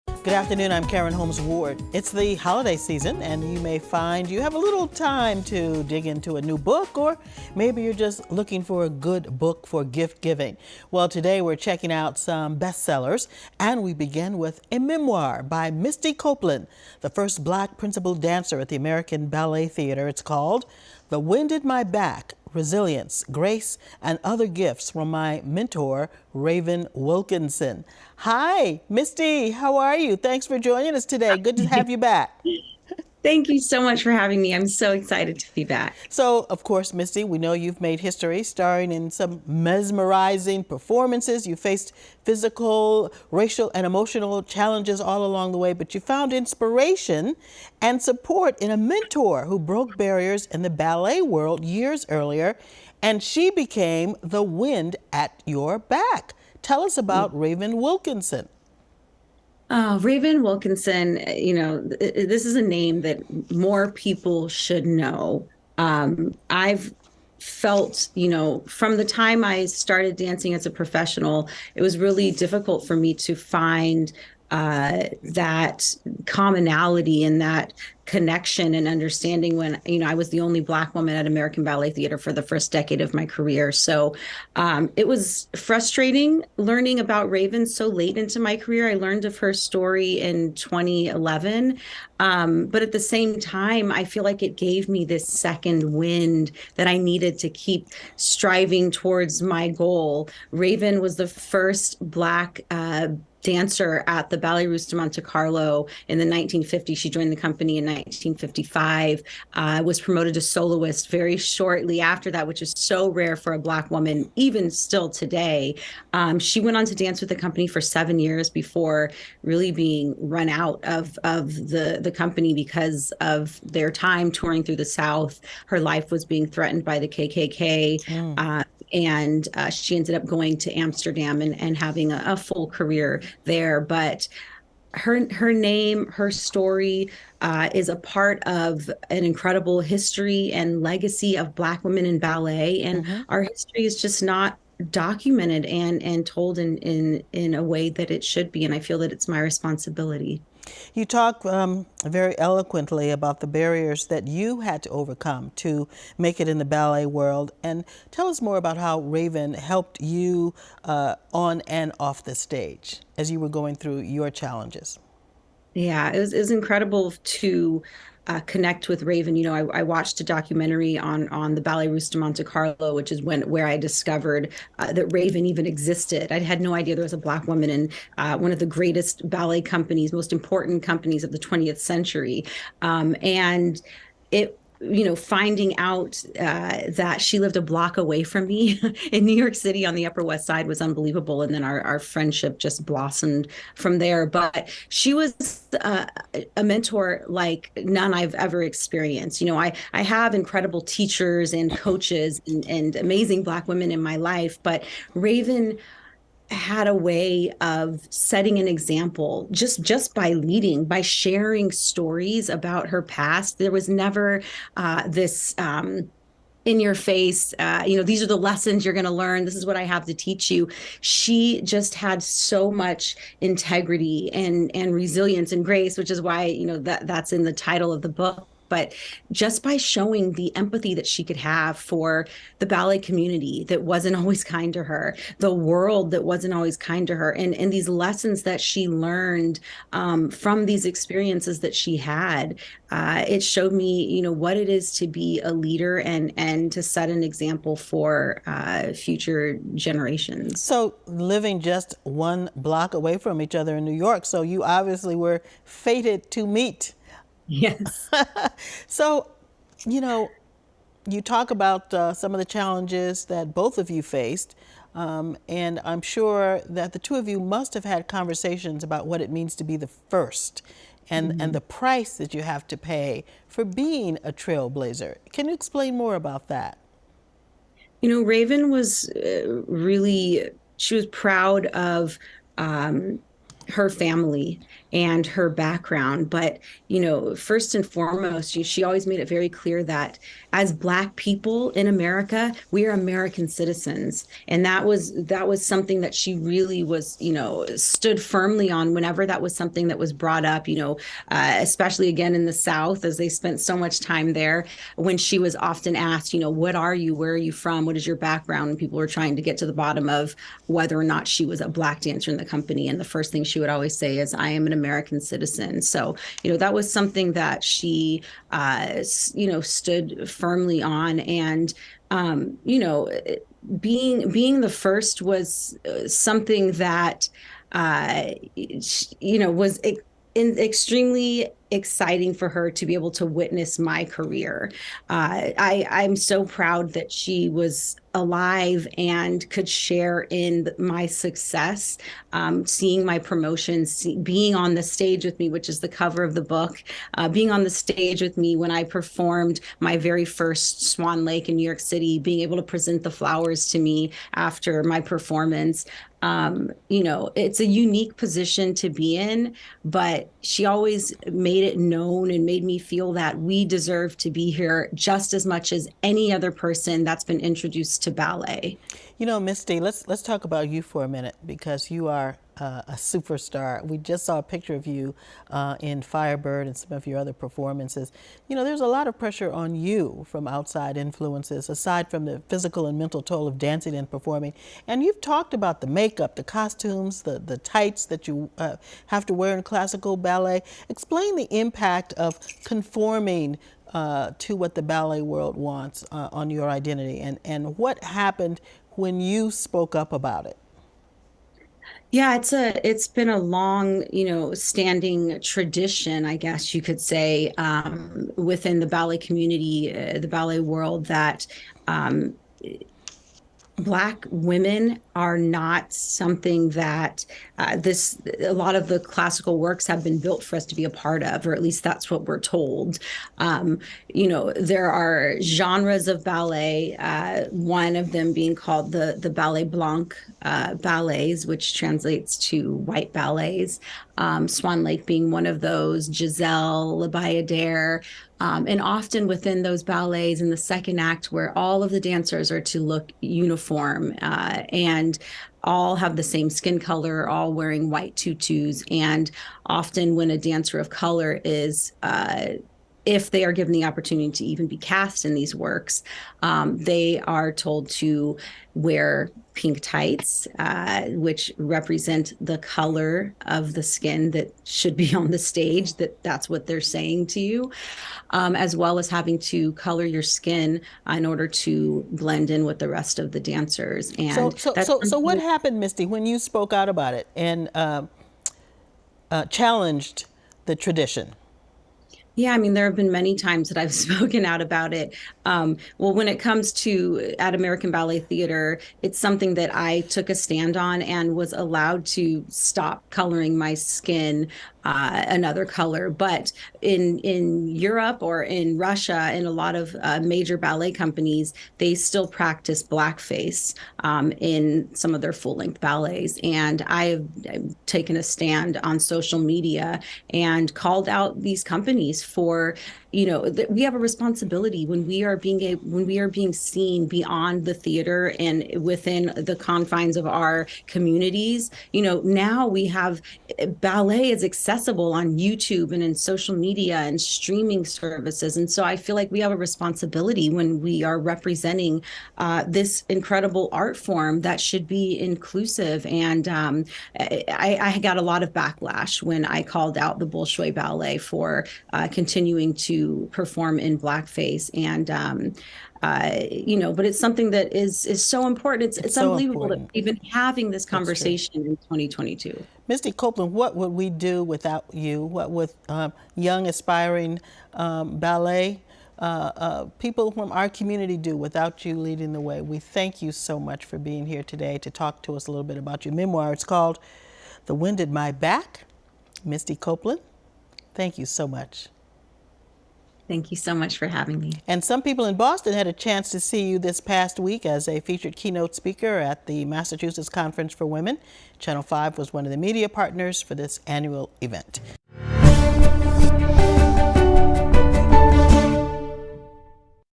Misty Copeland: The Wind at My Back
Misty Copeland, the first Black  principal dancer at the American Ballet Theater talks about her memoir "The Wind at My Back: Resilience, Grace, and Other Gifts from My Mentor Raven Wilkinson